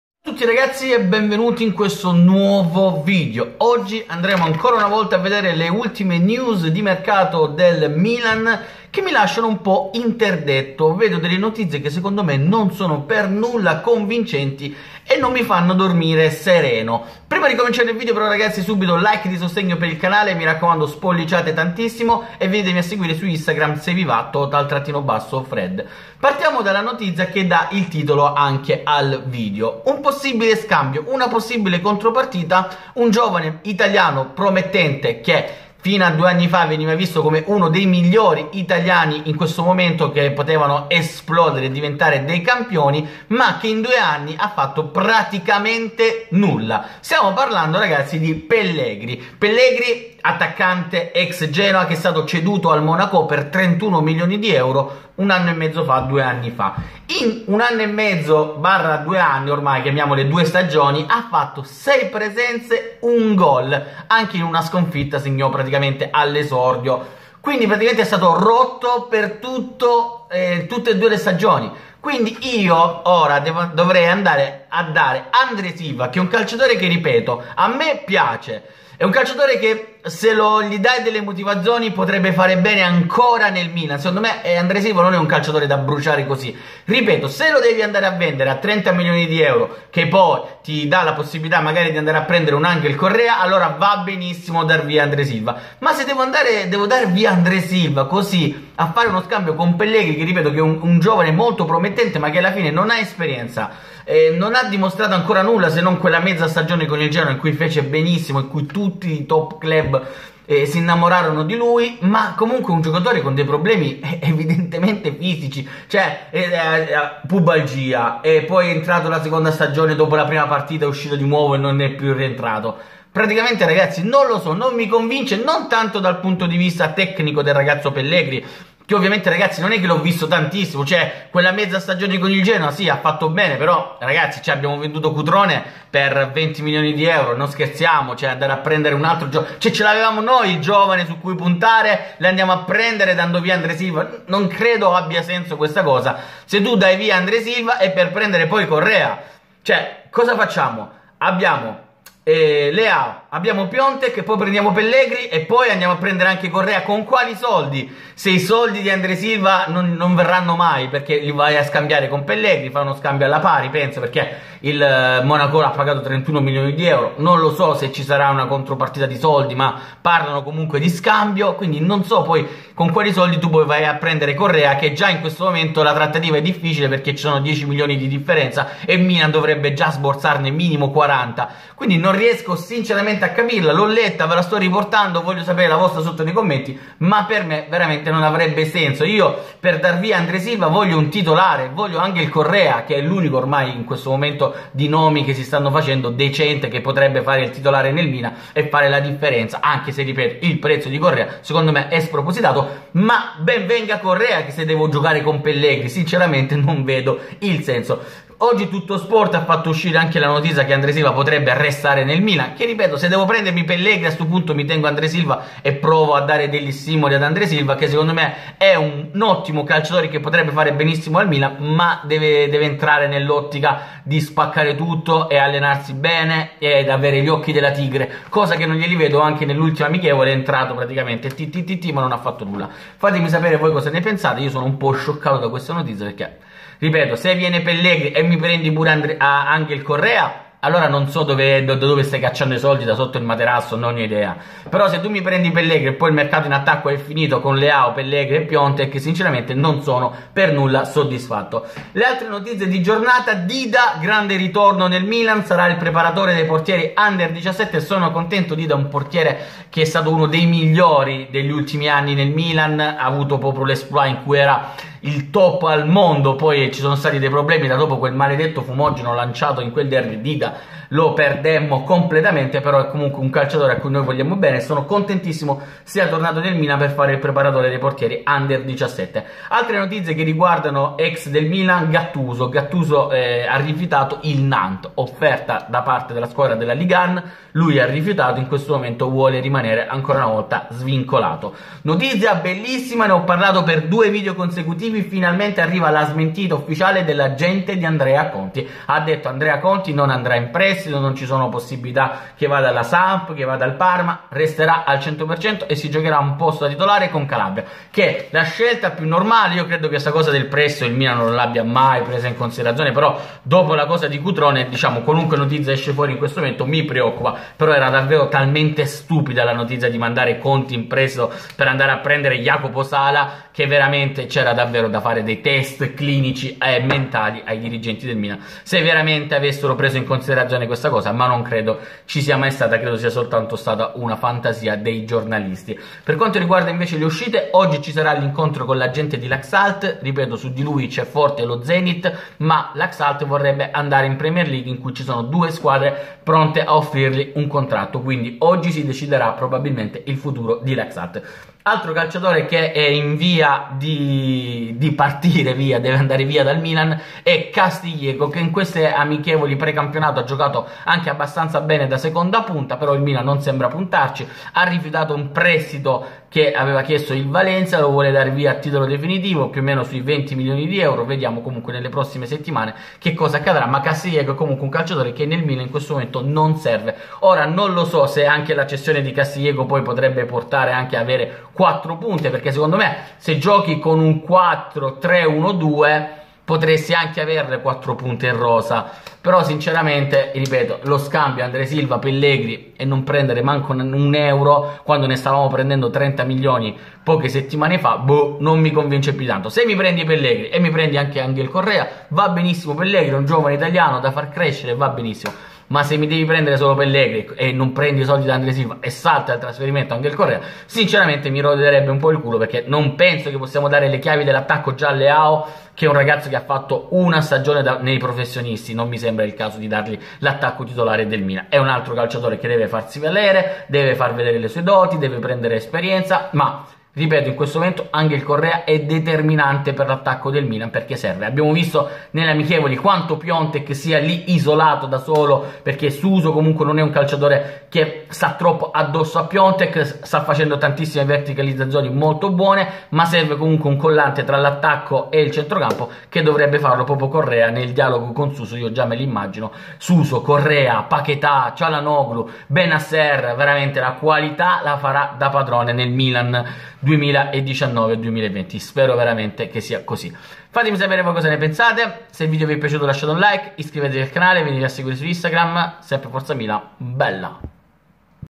UNO SCAMBIO SENZA SENSO! || News Milan
Lascia LIKE e CONDIVIDI il video

Possibile scambio tra Milan e Monaco con André Silva in Francia e Pellegri in rossonero. Scambio che non mi convince se dovesse far saltare il colpo Angel Correa